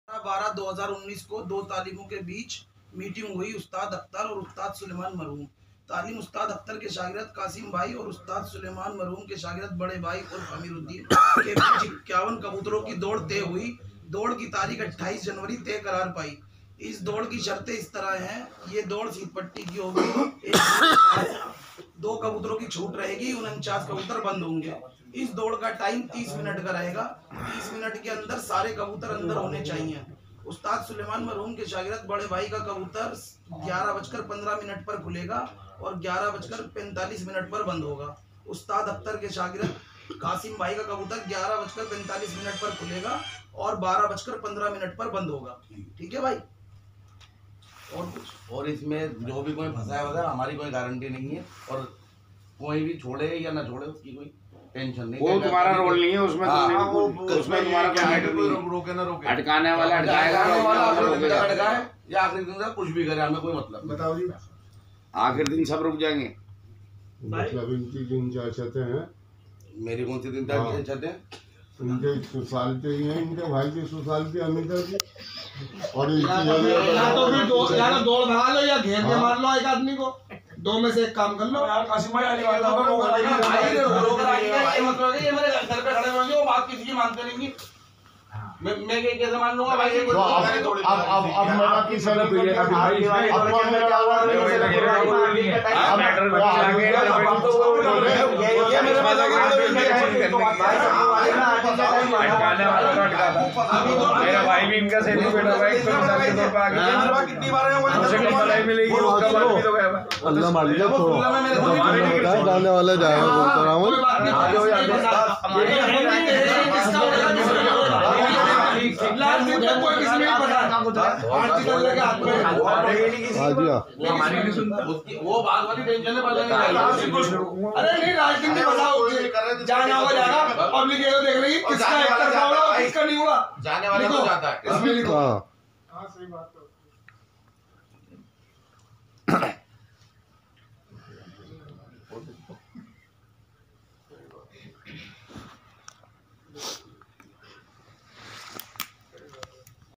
Ustad Akhtar Talim Vs Marhoom Ustad Suleman Talim
Pigeon Race - Qasim (Ustad Akhtar)  Vs Bade bhai,  Amiruddin (Ustad Suleman marhoom)  on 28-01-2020 at Gali Shahtara,  Delhi